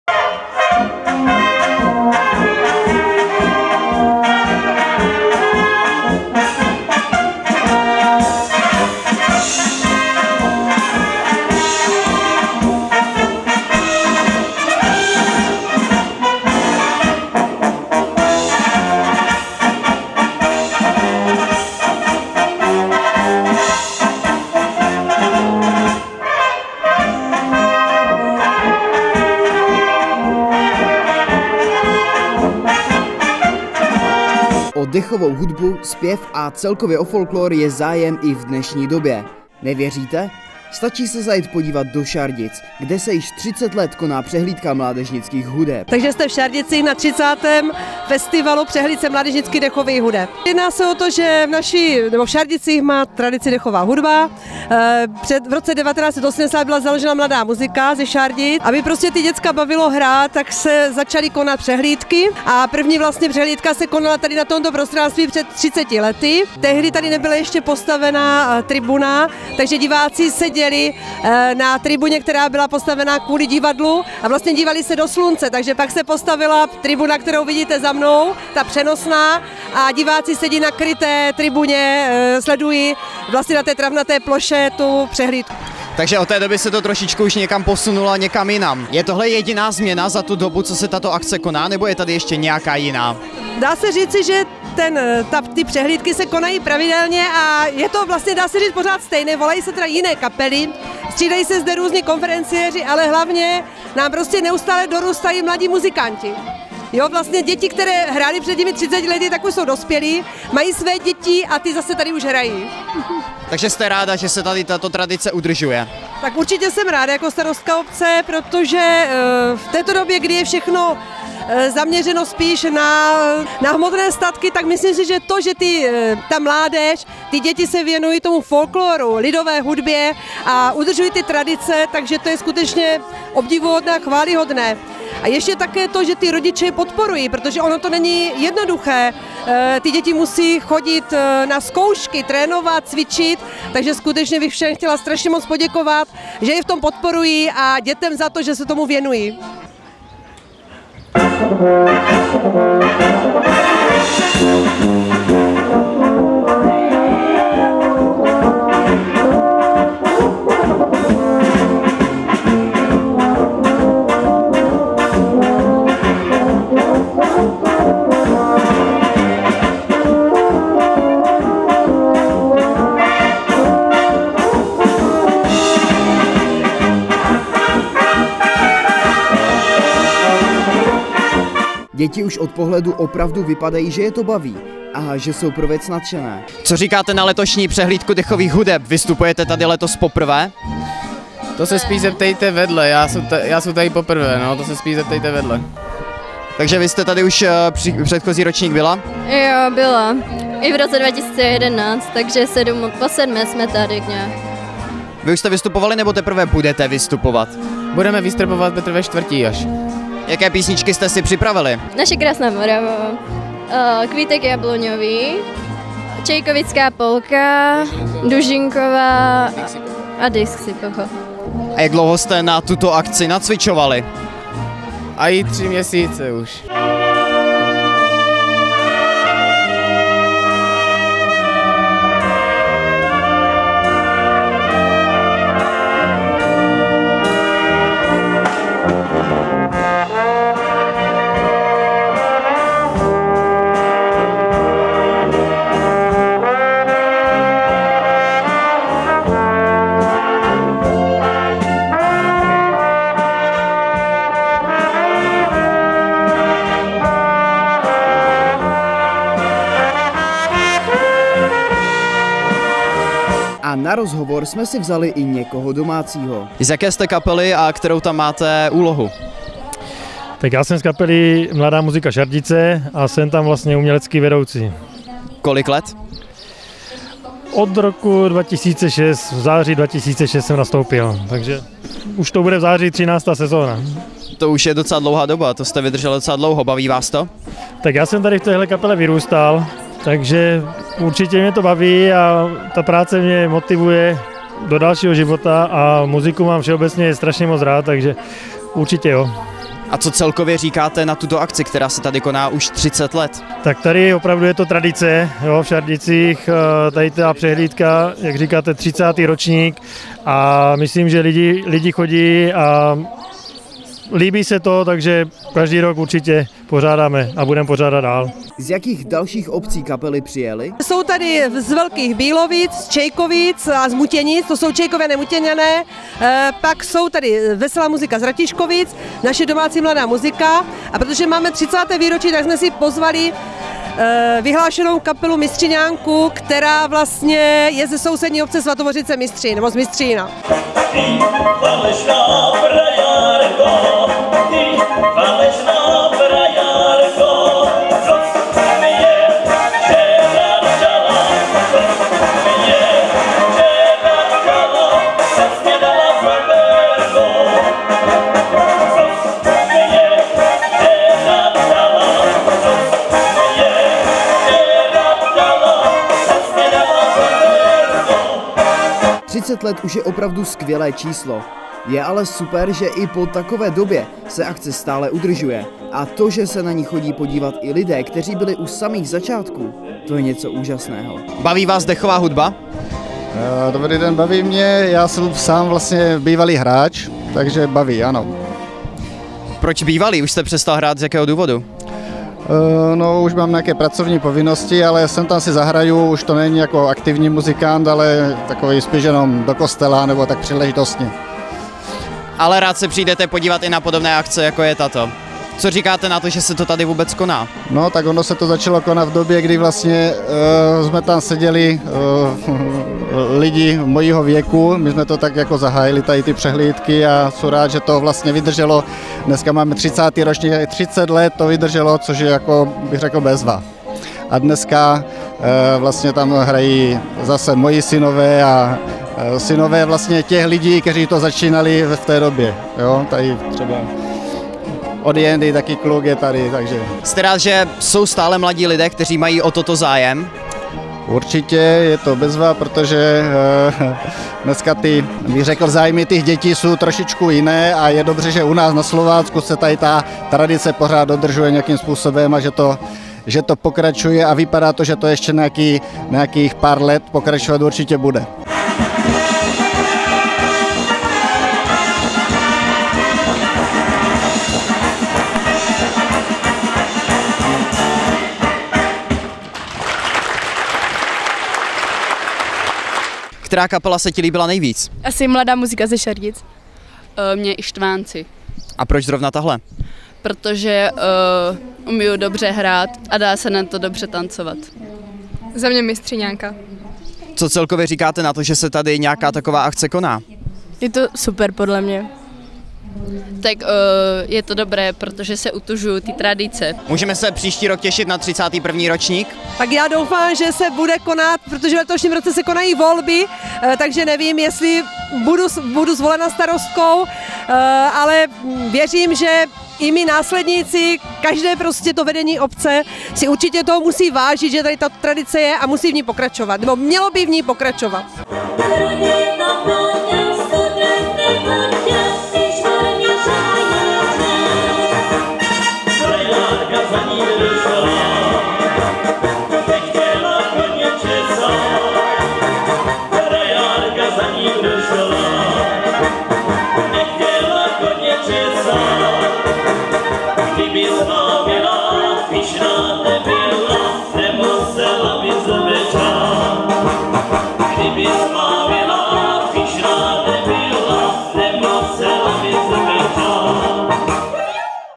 30. ročník Přehlídky mládežnických hudeb přivítal i Mistříňanku
O dechovou hudbu, zpěv a celkově o folklor je zájem i v dnešní době. Nevěříte? Stačí se zajet podívat do Šardic, kde se již 30 let koná Přehlídka mládežnických hudeb.